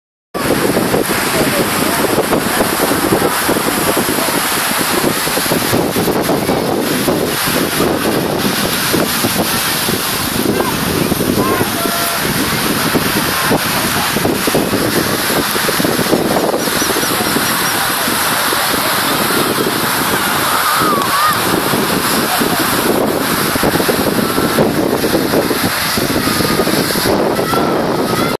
Rosignano Solvay Spiagge Bianche: kitesurfing
Kitesurfing alle spiagge bianche di Rosignano Solvay in Toscana (Italy).